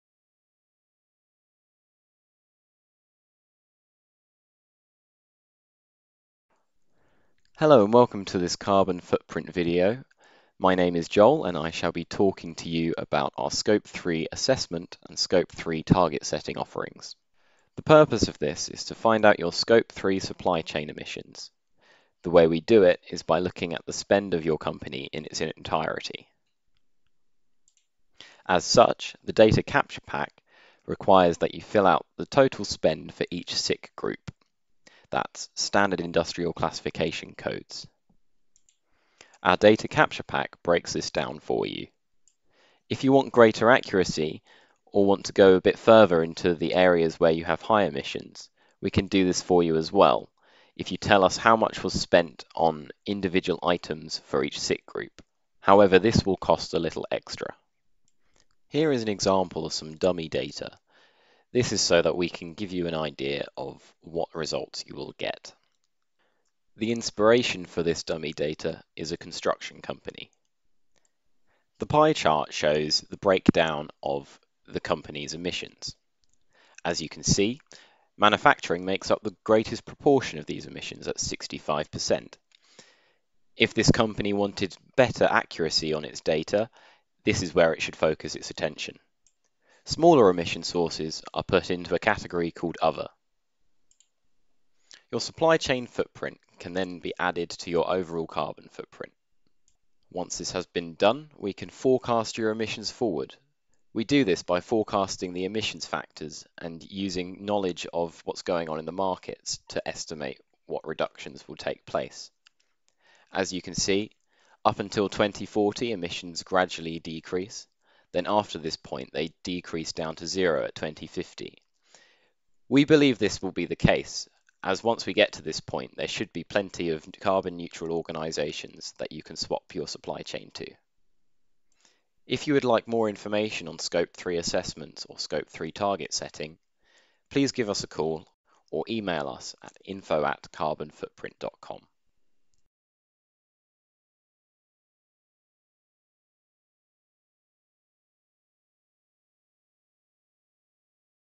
Scope 3 Assessment & Target Setting
How to assess your business's supplier related carbon emissions and to analyse to set targets for reduction, including for net zero carbon ambitions.